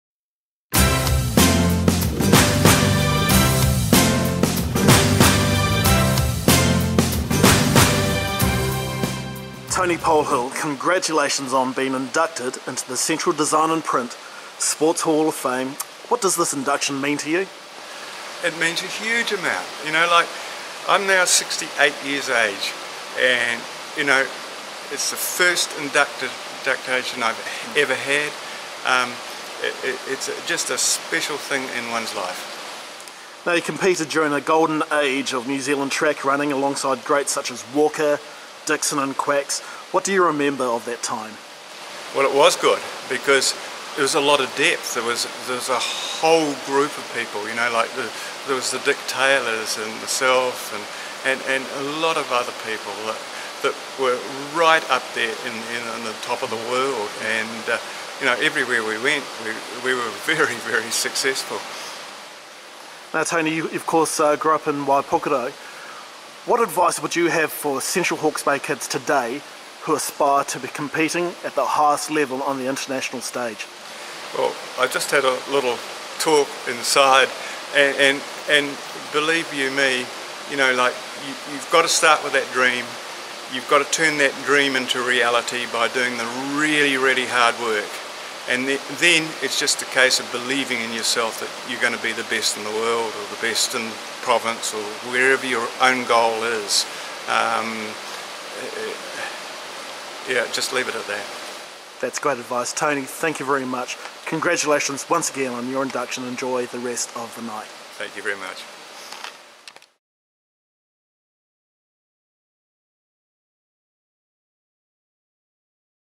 Central Hawke's Bay Sports Hall of Fame 2016 Inductee Tony Polhill
Tony Polhill, who competed in the 1500 metre final at the 1972 Munich Olympics, speaks after being inducted into the Central Design and Print CHB Sports Hall of Fame at the Central Hawk'e Bay Sports Awards 2016.